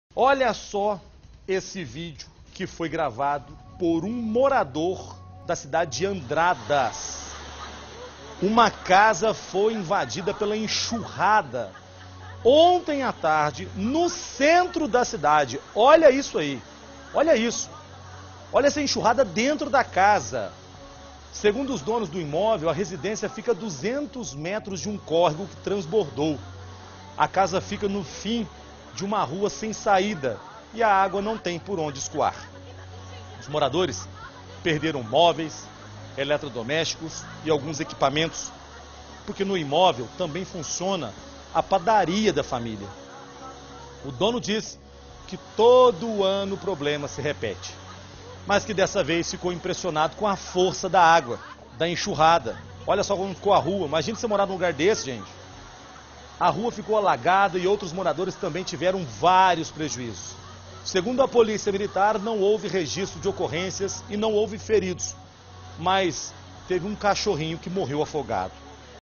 Chuva: córrego alaga e invade casa
Uma casa no centro de Andradas, no Sul de Minas, teve vários danos com a força da água da chuva. Uma correnteza se formou e levou o que tinha pela frente. As imagens impressionam e mostram a força da água.